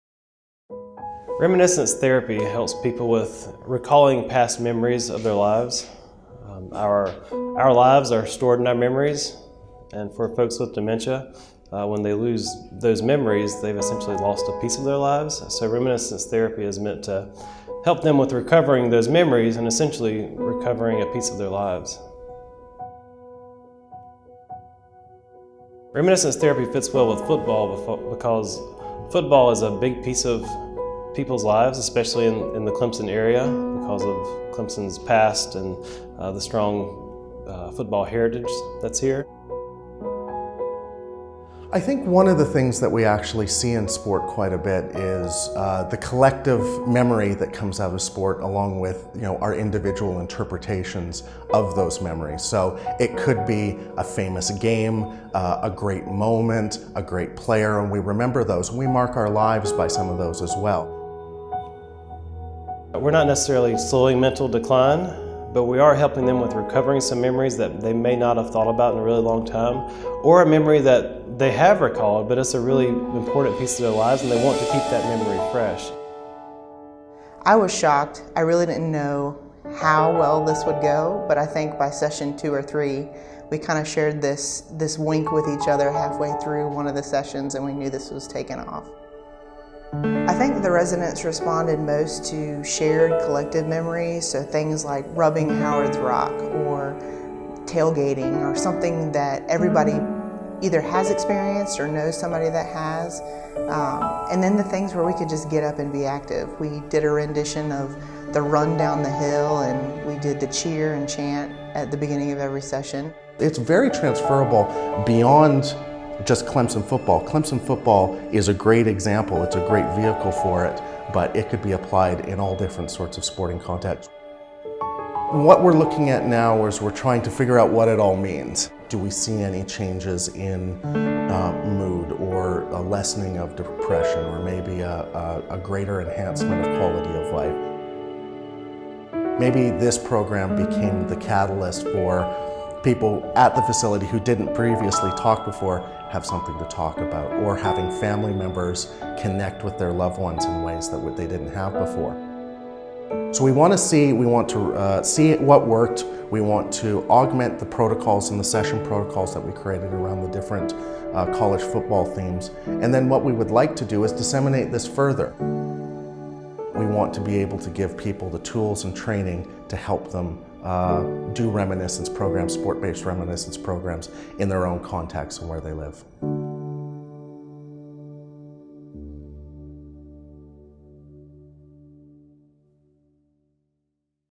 Clemson University Football Reminiscence Program
Researchers from Clemson University are using football to battle the persistent damage brought on by dementia. Faculty and graduate student researchers are studying the effectiveness of a reminiscence therapy program that uses memories associated with Clemson football to address many of the debilitating effects of dementia. The program uses football history, footage and memorabilia to provide a multisensory intervention for memory decline.